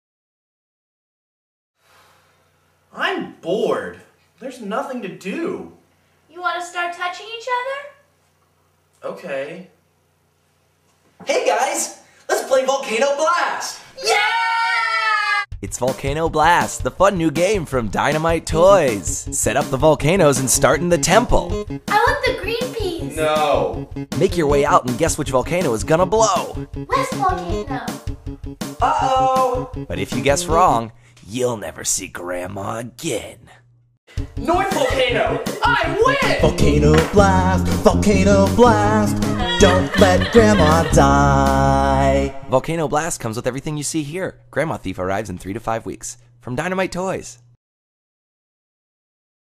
Volcano Blast!
Commercial for the new board game Volcano Blast!  Guard your loved one.

As seen in Matt Little's Big Show
Tuesday, July 31st
8:30 PM
Gotham City Improv
48 W. 21st St., 8th Floor
New York
$5.00